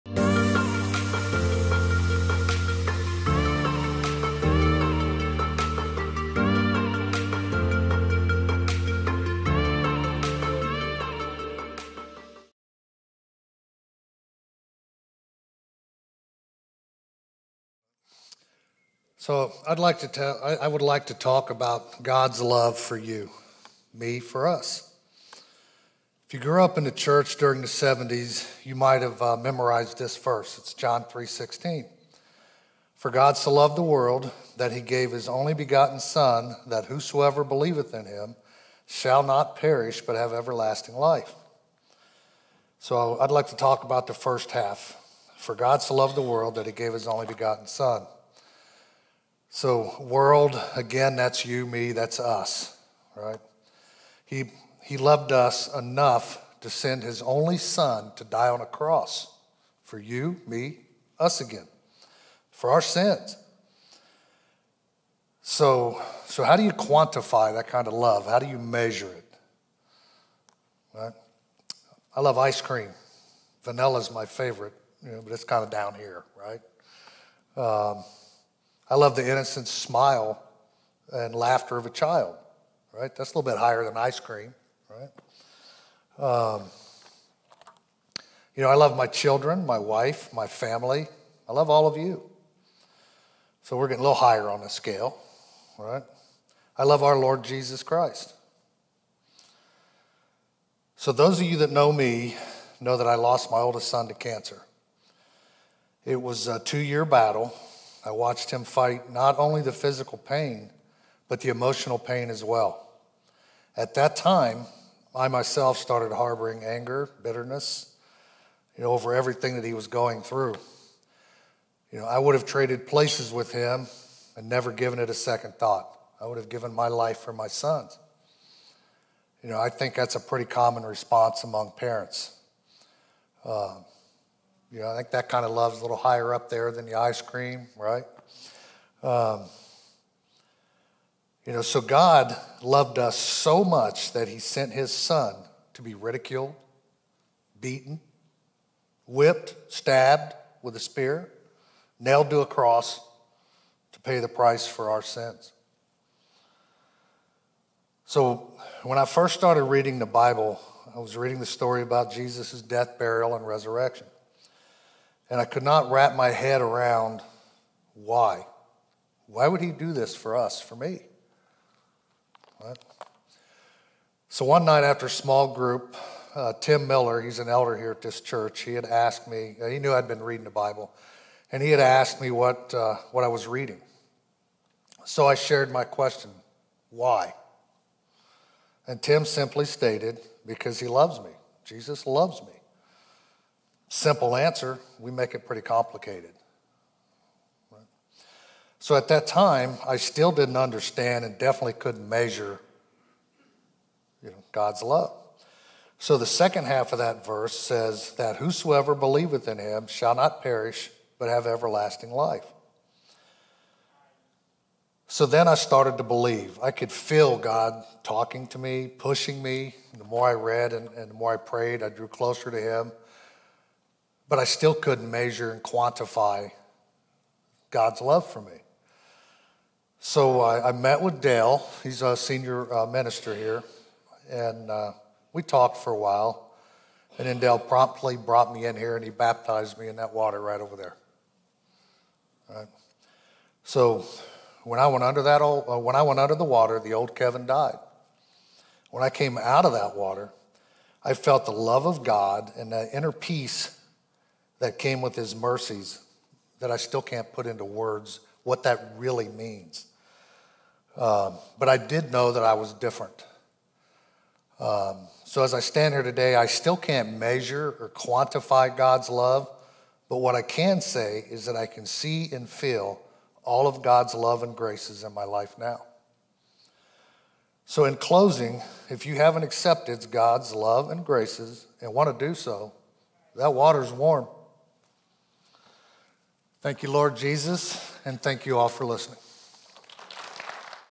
Grasping the Love of God | Kevin Reed
5x5 is a series where 5 speakers give their best 5 minute messages. Each speaker gets to choose their own topic and they only have 5 minutes to deliver their message before the timer ends.

Message: Grasping the Love of God
Speaker: Kevin Reed
Series: 5x5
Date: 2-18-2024